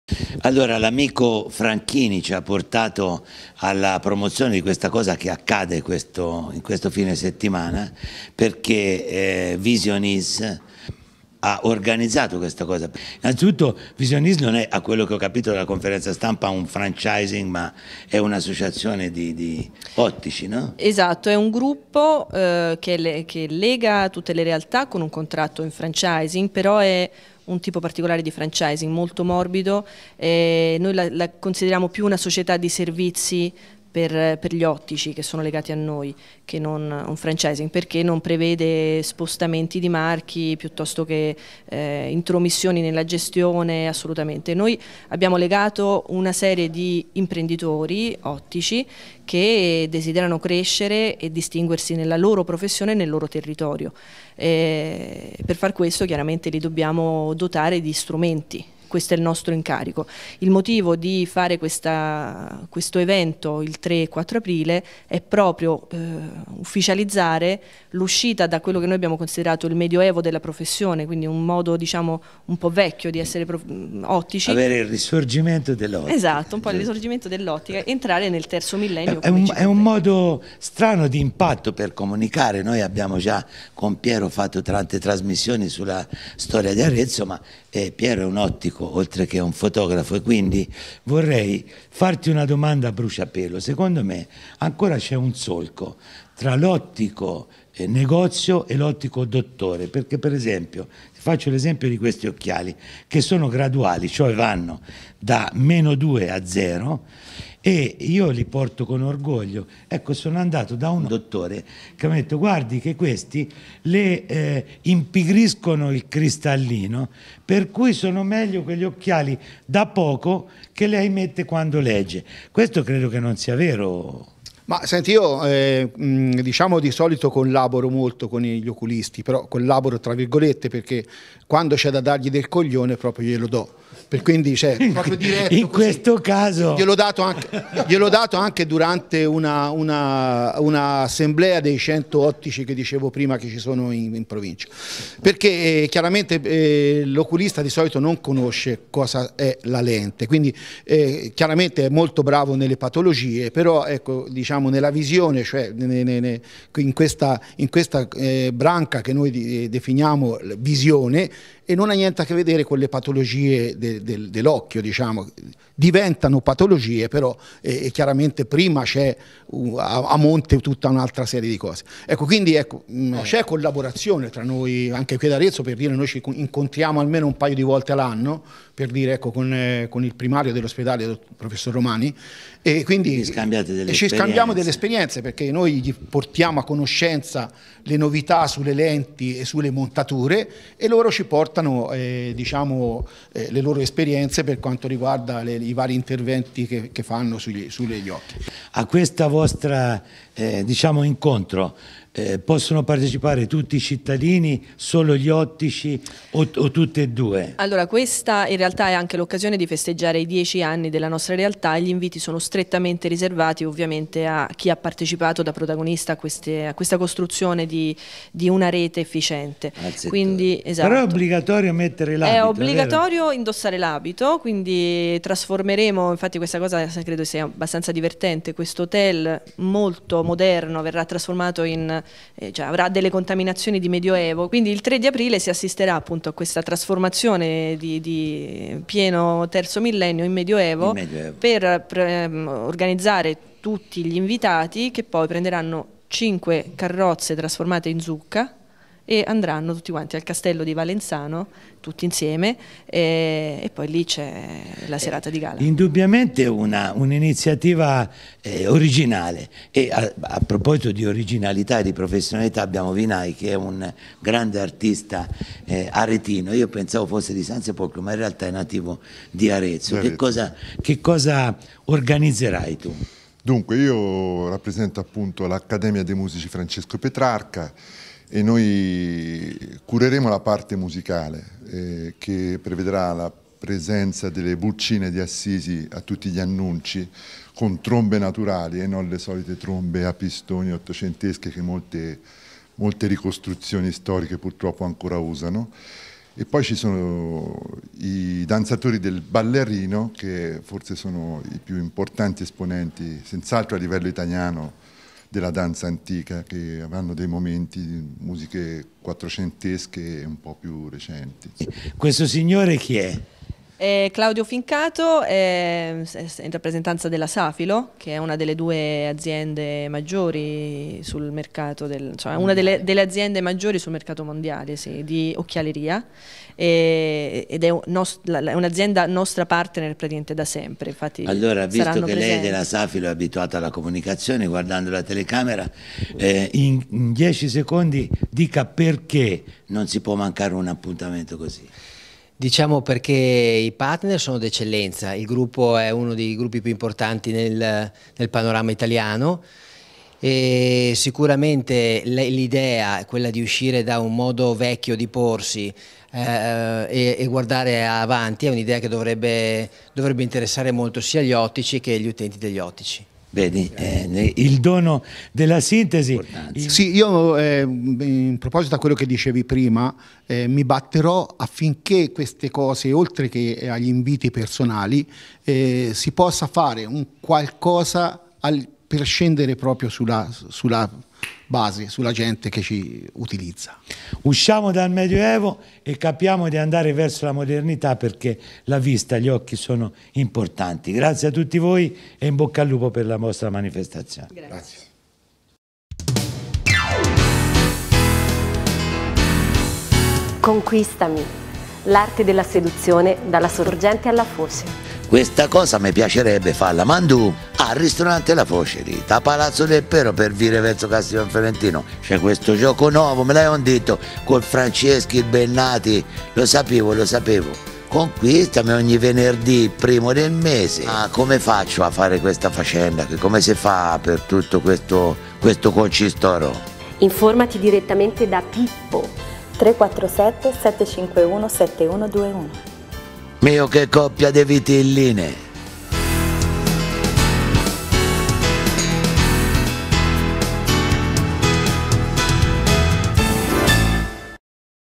CHIAROETONDO VISION IS GROUP
DOMENICA 3 APRILE VISION GROUP PER FESTEGGIARE I SUOI DIECI ANNI ORGANIZZA UNA CENA IN COSTUME MEDIOVALE AD AREZZO AL CASTELLO DI VALENZANO.
UN MODO PER INCONTRARE TUTTI  GLI OTTICI ASSOCIATI CHE VORRANO PARTECIPARE!